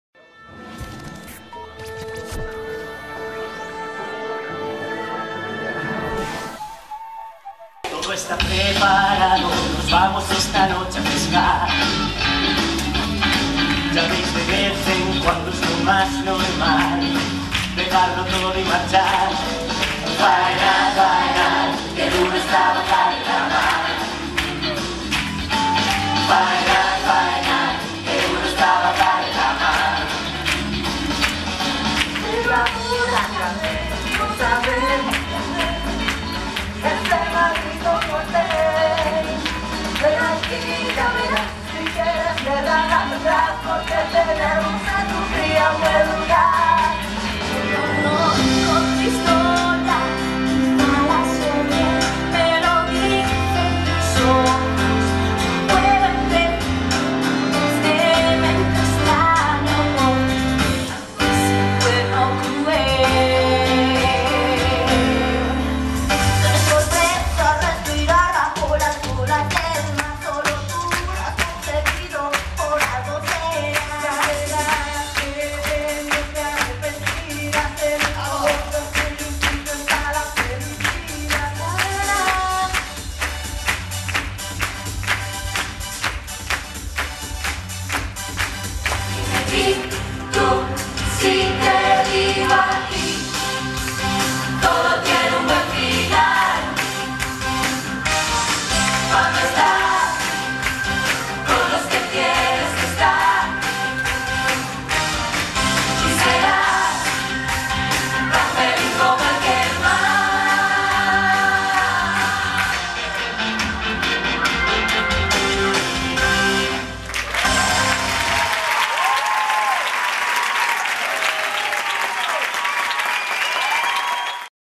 La Leyenda Del Unicornio (Highlights)
La nueva producción de Jana Producciones se estrena hoy y estará en cartel del 16 de noviembre de 2013 al 12 de enero de 2013. Sábados y domingos 12:30 horas. En el Teatro Fígaro de Madrid.